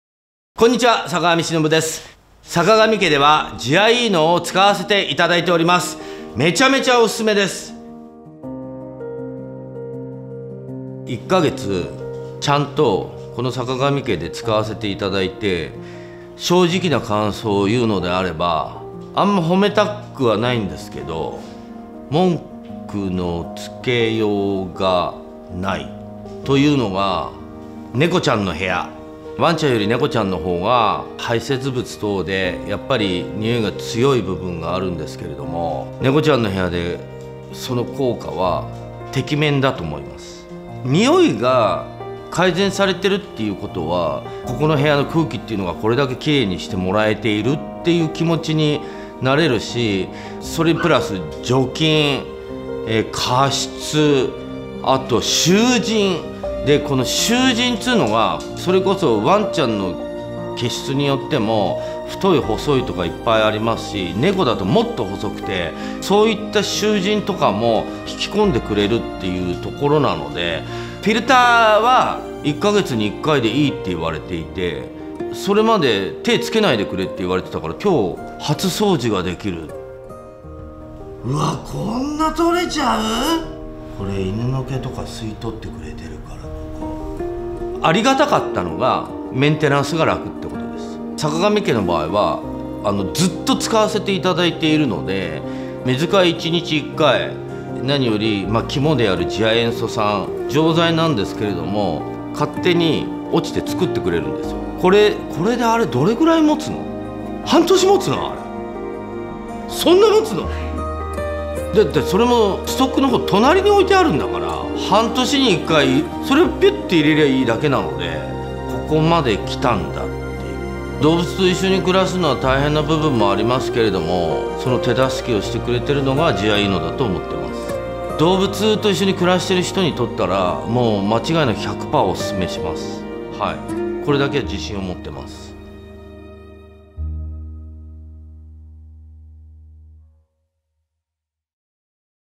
坂上忍さんが語るジアイーノ（完全版）【パナソニック公式】
動物保護施設「さかがみ家」で使われているパナソニックの「ジアイーノ」。
その効果を坂上忍さんにインタビューいたしました。

次亜塩素酸のチカラで除菌・脱臭。
さらにこれ1台で加湿や、花粉やハウスダスト・ペットの毛の集じんまでお任せ。

人もペットも、家族みんなが快適に暮らせる空気をお届けしたい。
パナソニックの空間除菌脱臭機「ジアイーノ」です。